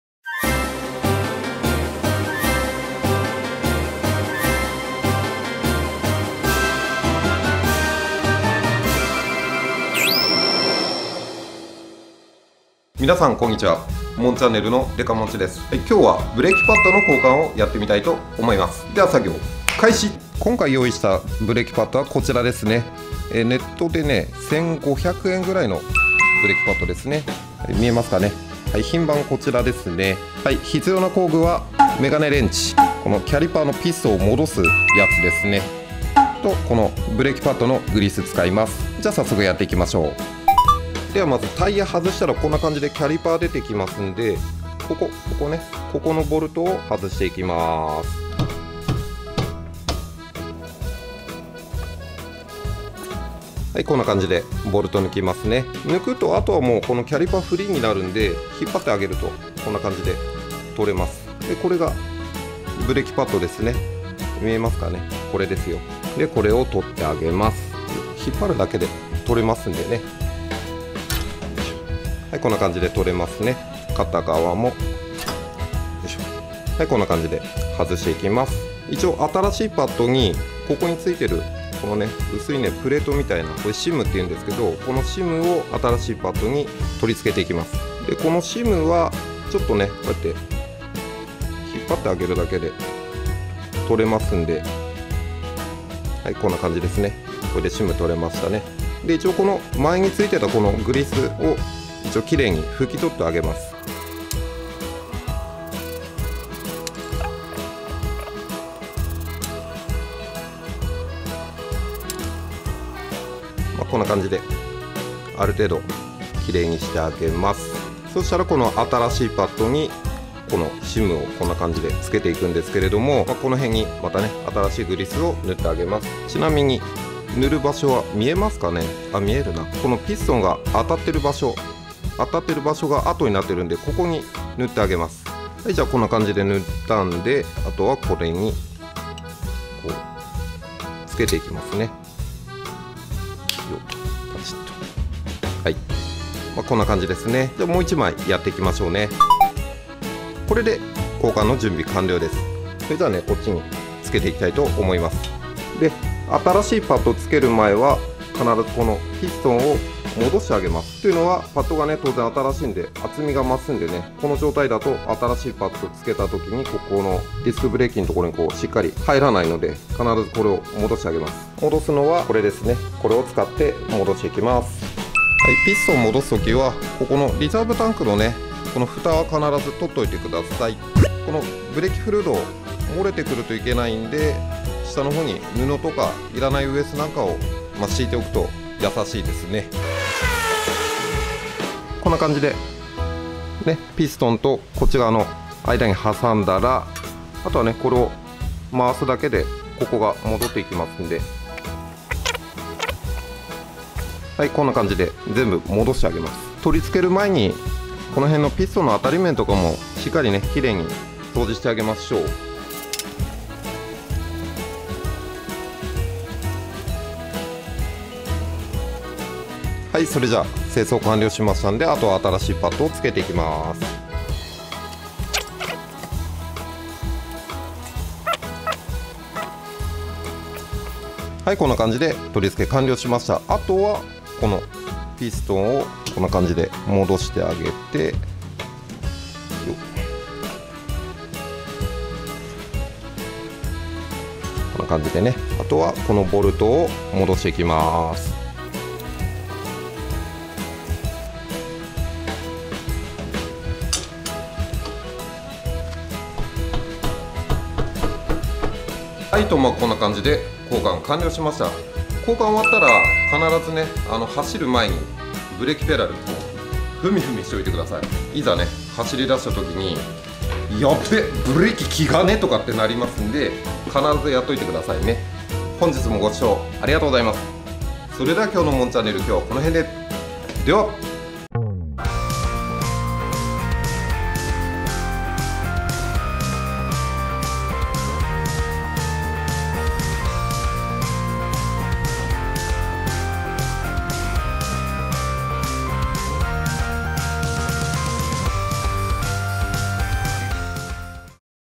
５分でわかる！ブレーキパッド交換【JB5 ライフ】
☆ほぼ毎週日曜日１９時なにかしら公開中☆
【もんChannel】

今回はブレーキパッド交換のやり方です♪

☆SNS☆
☆ブログ☆
【デカモンチのブログ】